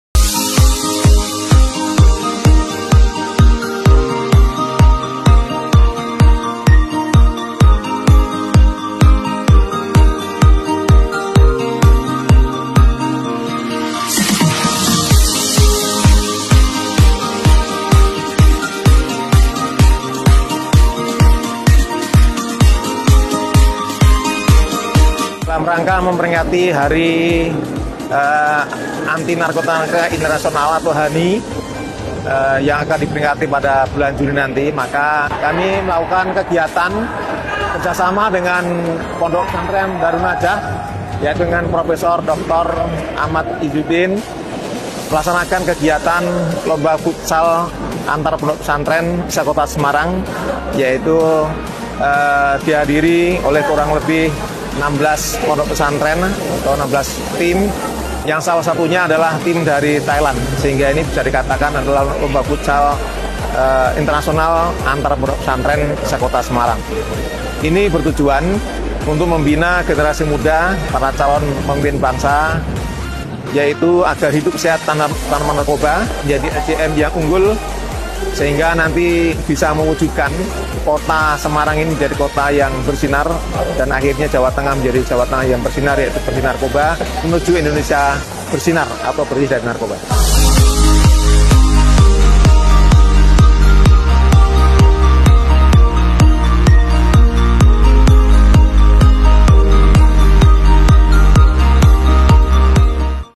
Kepala BNNP Jateng Membuka Turnamen Futsal Antar Pondok Pesantren se-Kota Semarang dengan sebuah GOL
GOL!!! Tendangan kaki kanan Kepala BNN Provinsi Jawa Tengah Brigjen. Pol. Dr. H. Agus Rohmat, S.I.K., S.H., M.Hum.melesakkan bola ke sudut kanan penjaga gawang yang dijaga oleh salah satu santri Pondok Pesantren Daarun Najaah diikuti gemuruh tepuk tangan dan teriakan peserta dan penonton. Tendangan Jenderal Bintang Satu tersebut tersebut dilakukan pada saat membuka Turnamen Futsal Antar Pondok Pesantren se-Kota Semarang di GOR Udinus Sport Center pada Kamis, 23 Mei 2024.
Dalam Sambutannya, Kepala BNNP Jateng memberikan penghargaan yang setinggi-tingginya kepada Pesantren Life Skill Daarun Najaah yang telah berkomitmen nyata dalam membantu upaya penanganan masalah narkoba di Indonesia khususnya di Kota Semarang.
Dan di akhir sambutannya, Kepala BNNP Jateng mengapresiasi kegiatan ini karena merupakan implementasi konkrit melalui kampanye anti narkoba pada kegiatan Turnamen Futsal Antar Pondok Pesantren. Serta dapat menjadikan pesantren BERSINAR.
"Bisa kita lihat inilah sinergi sesungguhnya, BNNP Jateng, Pondok Pesantren Daarun Najaah, dan Universitas Dian Nuswantoro bekerjasama mewujudkan Jateng Bersinar" jelas Jenderal Bintang Satu tersebut.
"Ketika semua lapisan masyarakat bergerak bersama maka bahaya narkoba akan kita bisa atasi juga bersama-sama dengan mudah"imbuh beliau.
Sementara itu Pengasuh Pesantren Lifeskill Daarun Najaah Prof. Dr. K.H. Ahmad Izzudin, M.Ag.sangat mengapresiasi dukungan BNNP Jateng dalam turnamen ini. "Beliau Kepala BNNP Jateng bisa menjadi contoh santri juga bisa berprestasi dan menjadi pejabat di pemerintahan".
Turnamen ini diikuti oleh 15 tim pesantren dari Kota Semarang serta 1 tim futsal mahasiswa asing dari Thailand. Adapun para pemenang turnamen ini adalah sebagai berikut:
Juara I: Ponpes An Nashimiyah dari Kelurahan Bojongsalaman, Kecamatan Semarang Barat.
Juara II: Ponpes Al Ma'rufiyah dari Ngaliyan Semarang.
Juara III: Ponpes MM Al Amaliyah dari Ngaliyan Semarang.
Turnamen ini menjadi bukti nyata upaya bersama dalam mengampanyekan gaya hidup sehat dan bebas narkoba di kalangan santri.